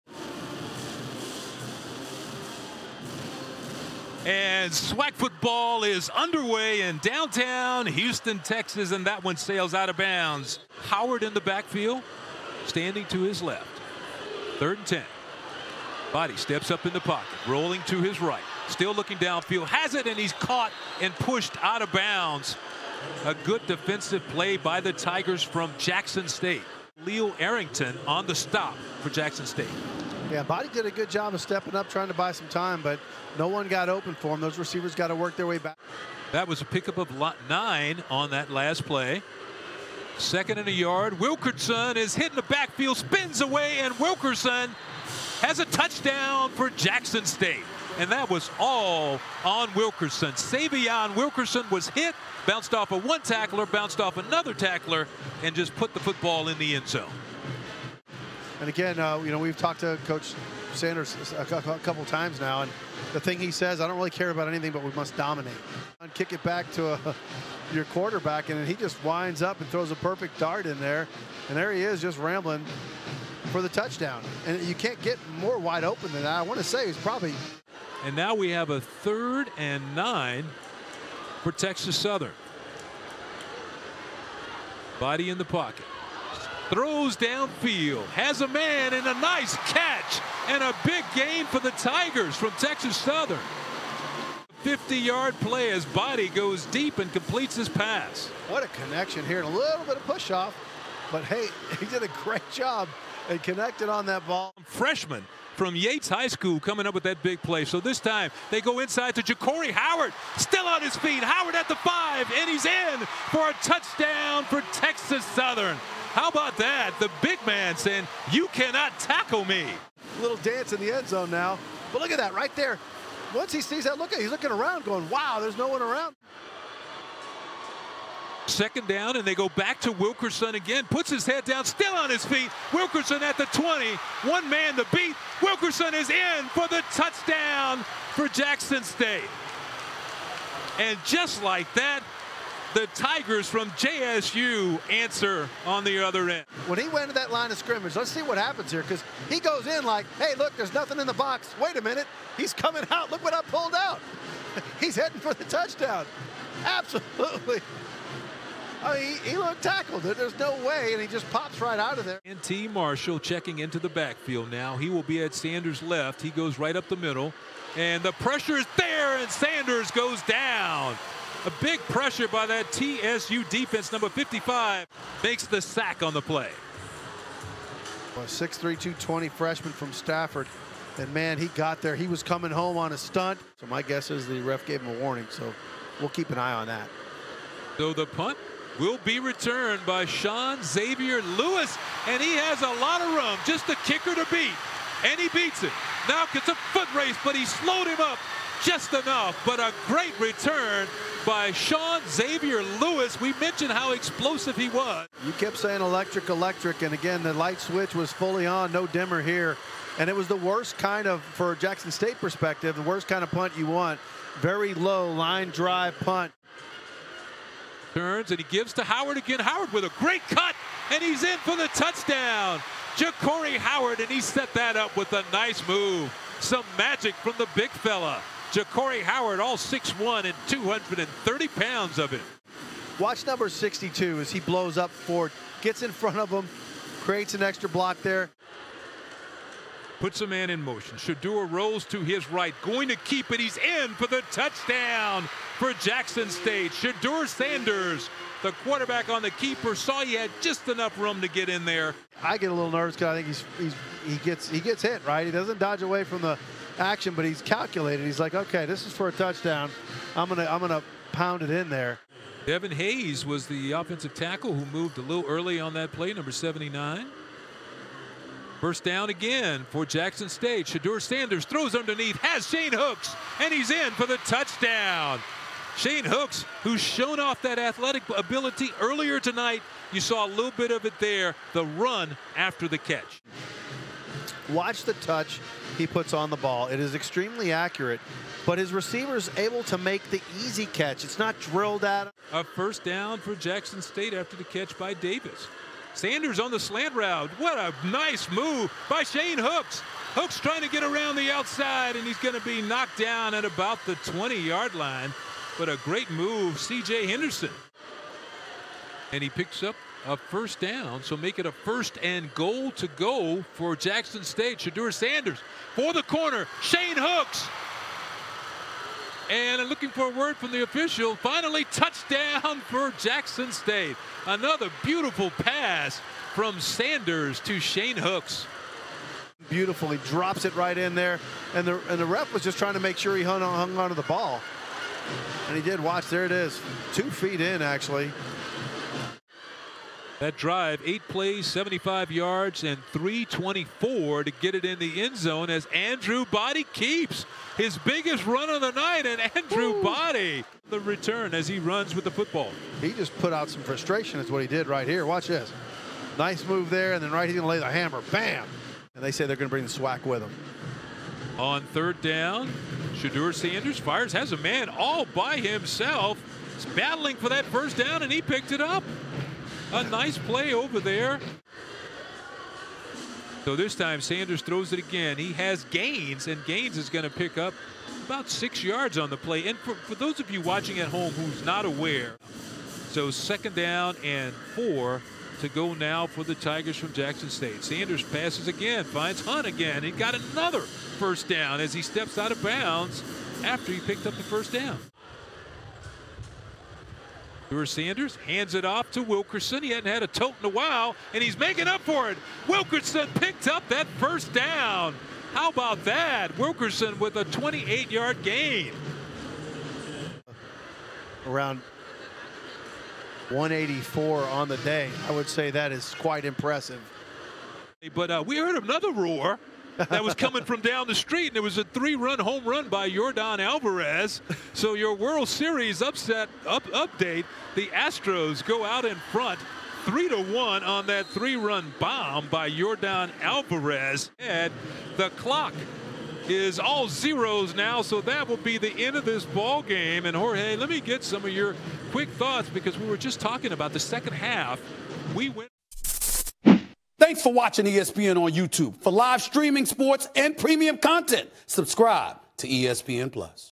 Jackson State Tigers vs. Texas Southern Tigers | Full Game Highlights
The Jackson State Tigers defeated the Texas Southern Tigers, 41-14, with Shedeur Sanders throwing for 252 YDS & 3 TDs for Jackson State.